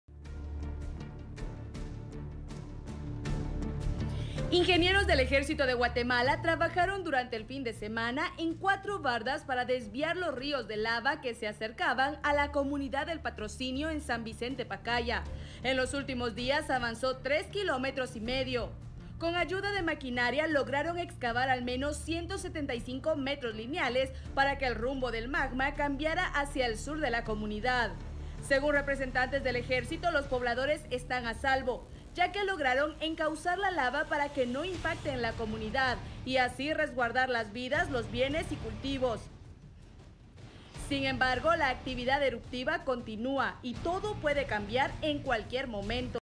Desviaron recorrido de lava del volcán Pacaya
Realizan trabajos de excavación para que la lava se movilice hacia otro lugar

El volcán de Pacaya ha registrado bastante actividad durante los últimos dos meses, avanzando la lava rápidamente hasta el punto que se encuentra cerca de llegar a las poblaciones que se encuentran aledañas al coloso.